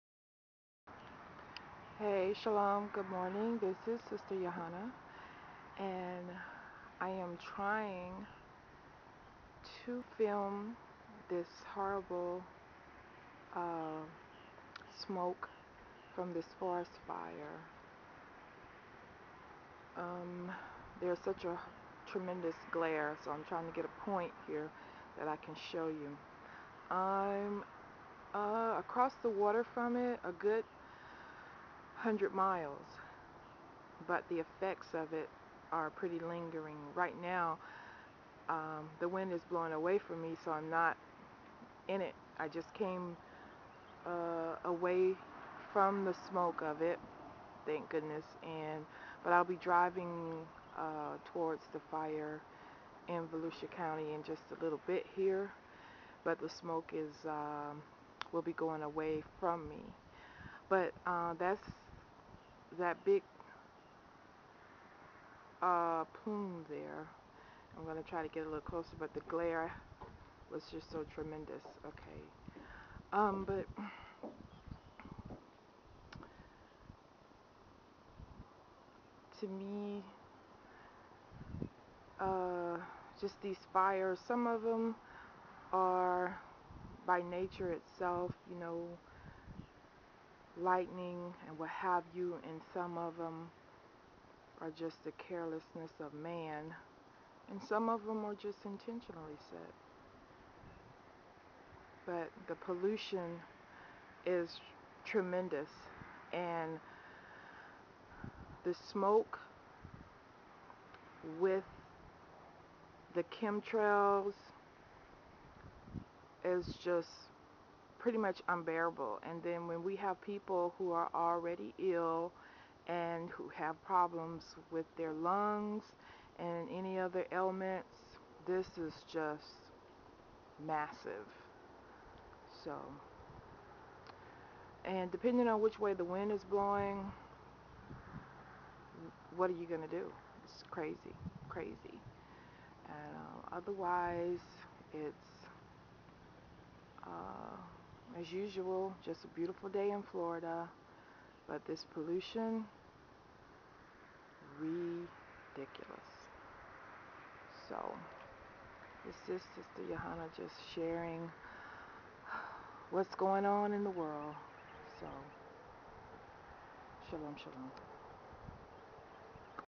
Fires in Florida
Fire, Smoke and Pollution in Florida...What can we do. There is no where to go...we have to breathe air.